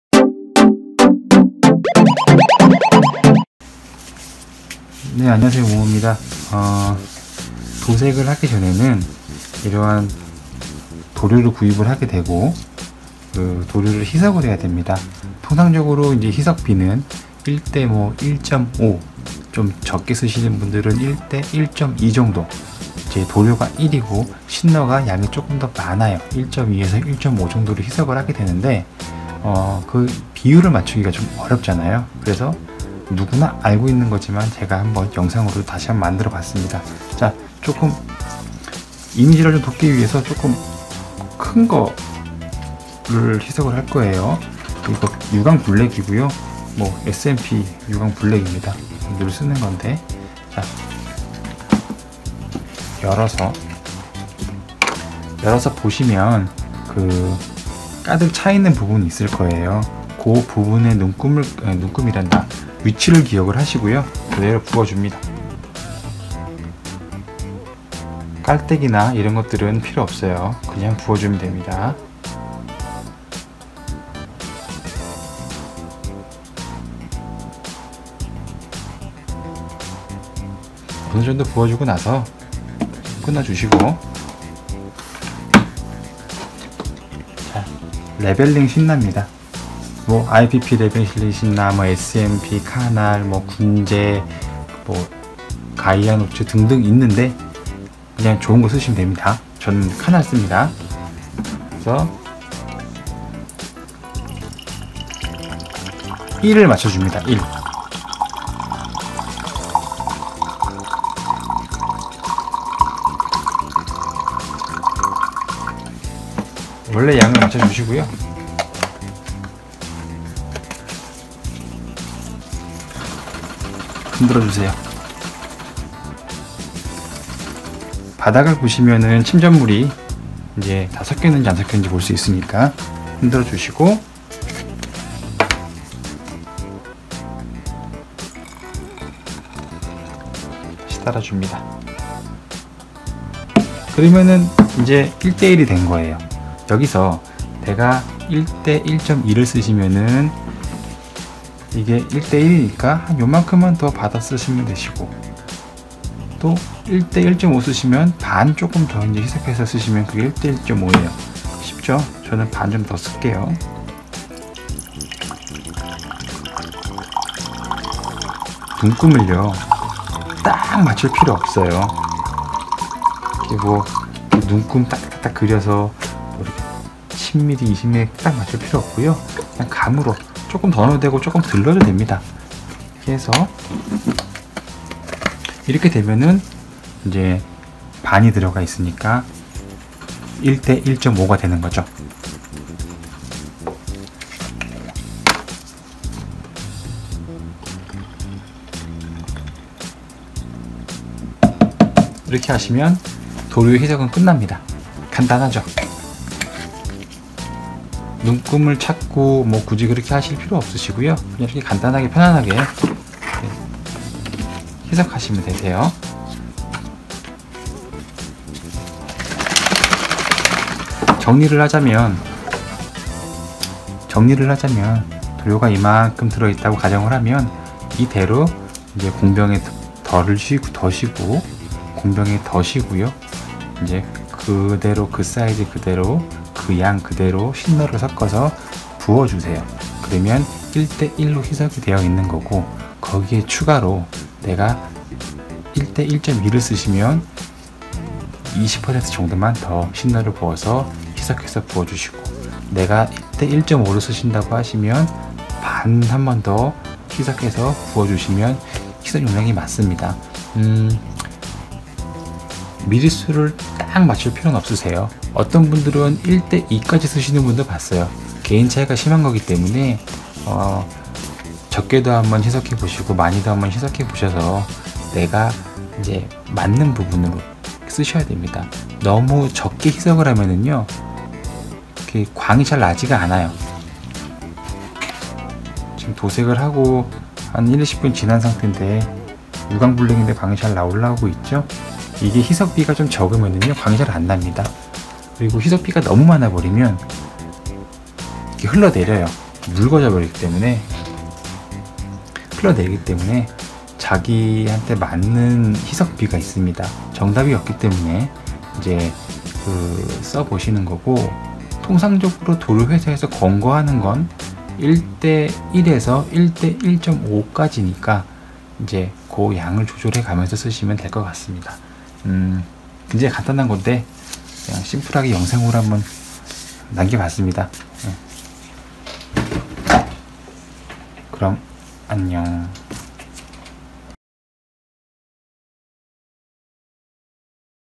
[모모] 도료를 공병(희석병)에 간단하게 희석하는법 (건담도색)
안녕하세요. 모모입니다.

도료를 구입하고 희석하는 방법에 대해서 간단한 영상을 만들어봤습니다. 다 알고 계시는 내용이지만 영상을 만드는것이 초보인만큼
간단한것 부터 만들어봤습니다.

부족하지만 많은 관심 부탁드립니다.

구독도 부탁드립니다!